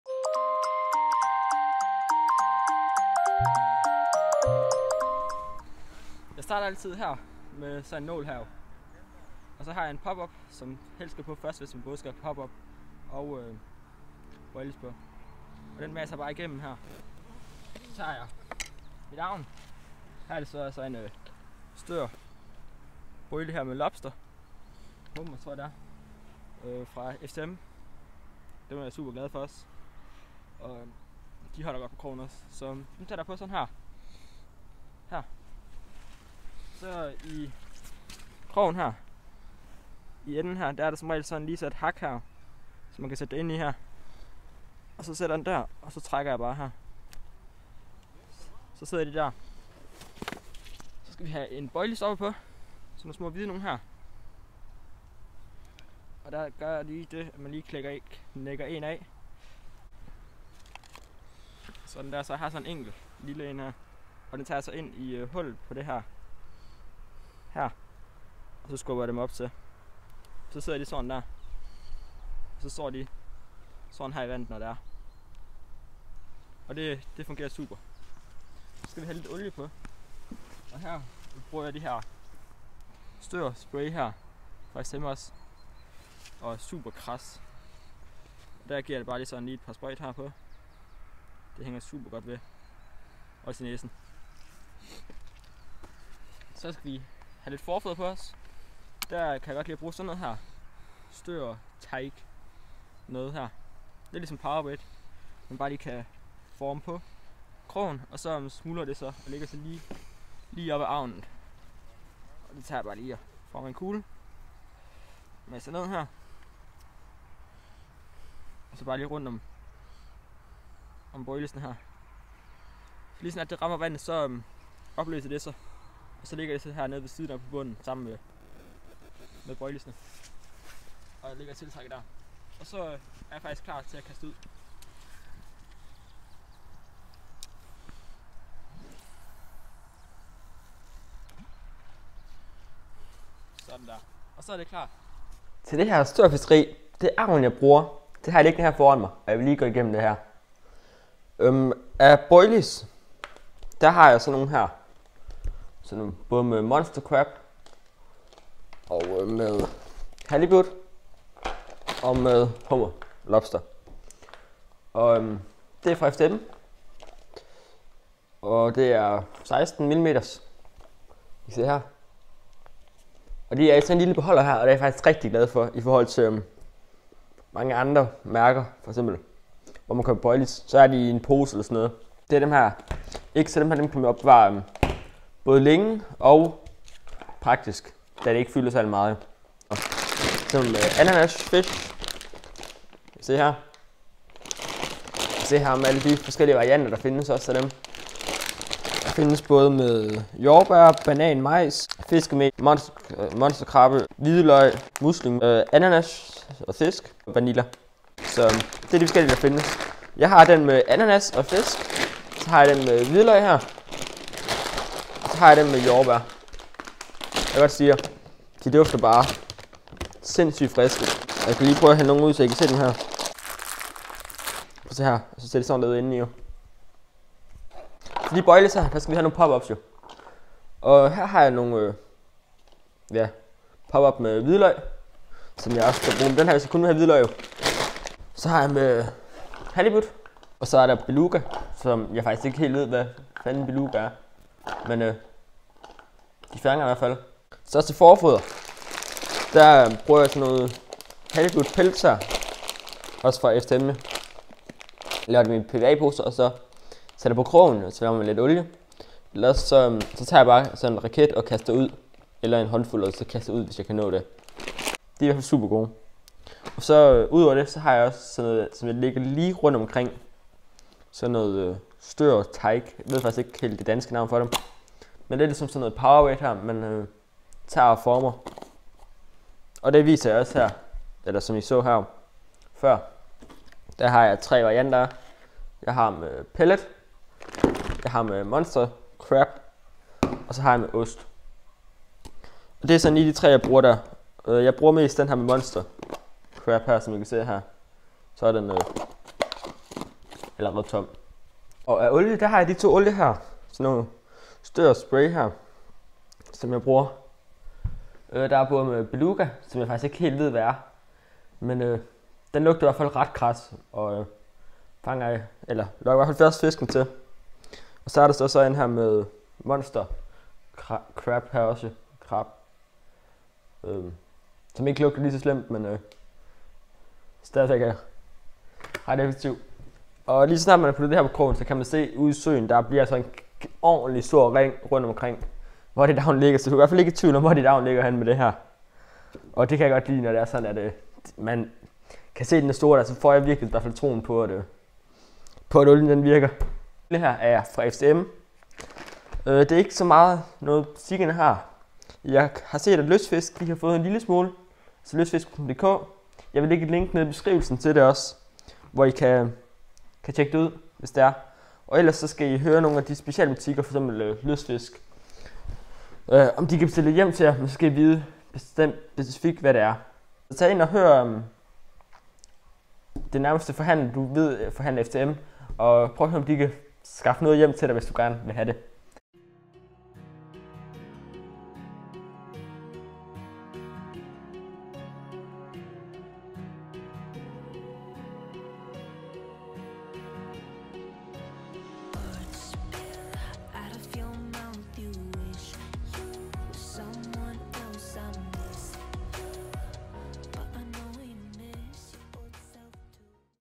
Størfiskeri - Sådan gør du og hvilket agn bruger jeg
Stør agn jeg bruger

Del gerne video og hjælp mig med at komme ud til flere personer
Kontakt mig på denne mail hvis du vil sponsorere en video 😉